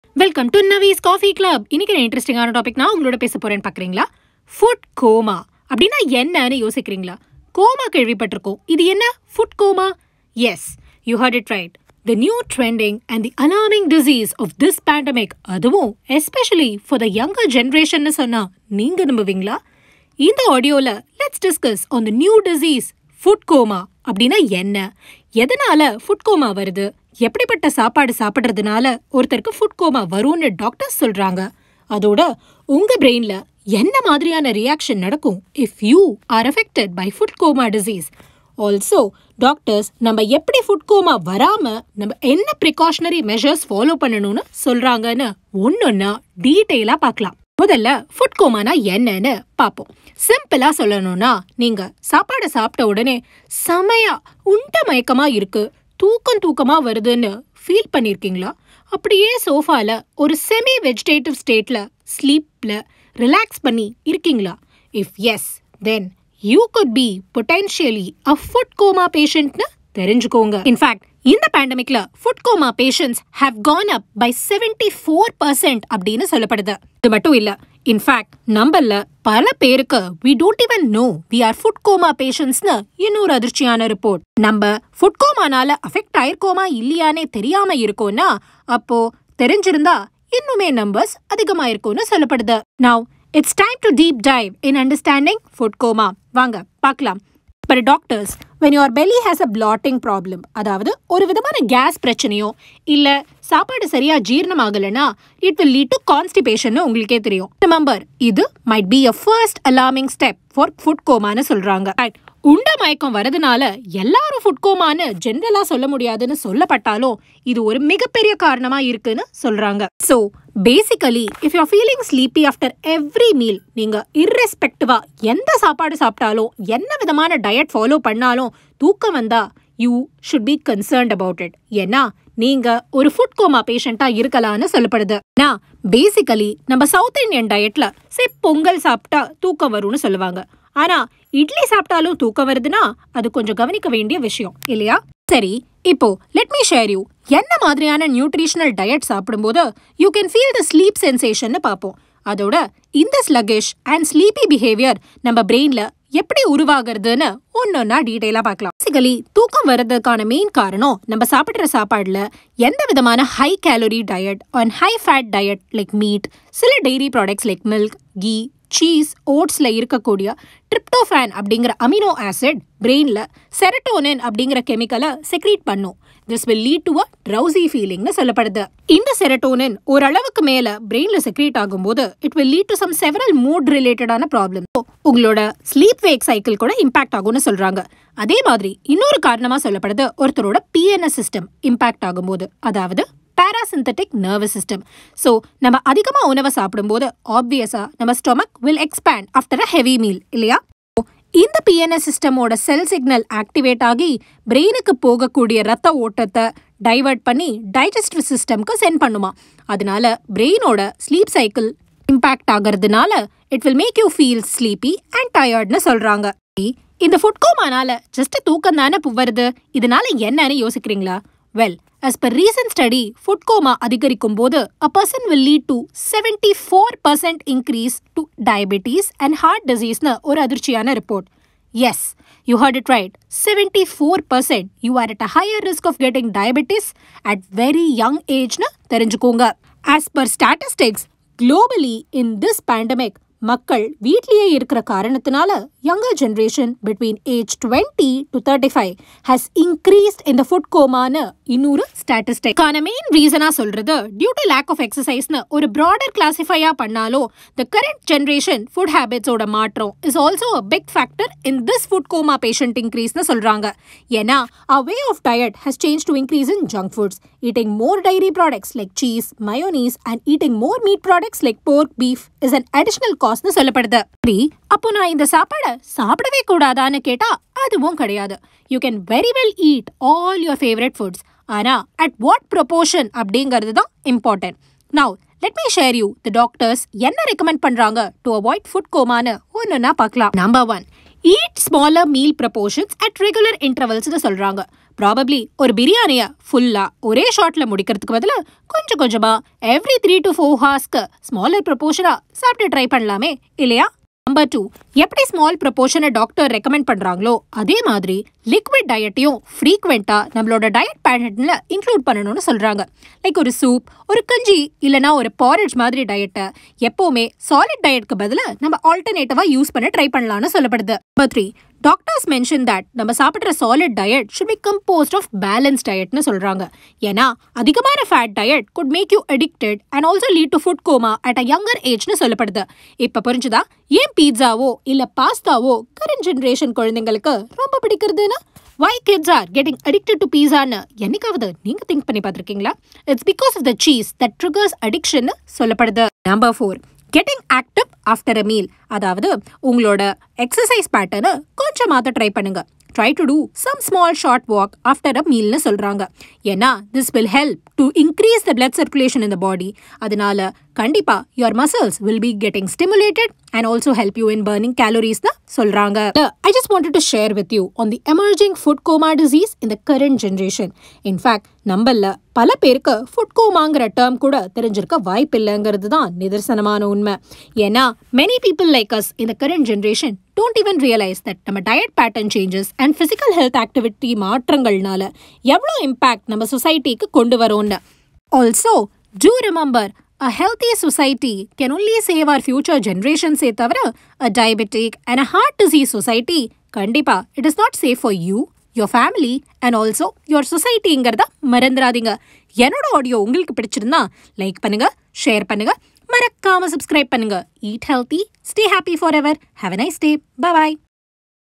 NaVi Coffee Club|Food Coma - Explained|Emerging Pandemic disease|Health Effects|Precautions|Tamil
This audio discusses in detail about the Food Coma - The emerging pandemic disease in young generations. Let’s dive deep into understanding about this ‘Food Coma’, how does a person get this food coma disease, what reaction happens to your brain when you are affected with this food coma disease and it’s science related to it. Also, the shocking statistics anoug the numbers reveal that 74% of younger generation is affected with food coma in this pandemic. What does this alarming numbers about food coma imply to you ? What do doctors recommend as a precautionary measure to prevent from being food coma patient.